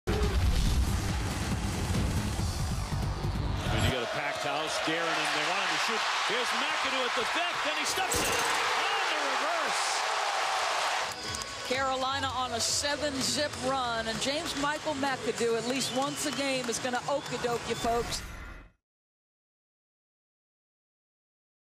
UNC James Michael McAdoo Goes Reverse Dunk On Break - ACC Must See Moment
2/23/13: Reverse jam by James Michael McAdoo and the Dean Smith Center goes wild!!!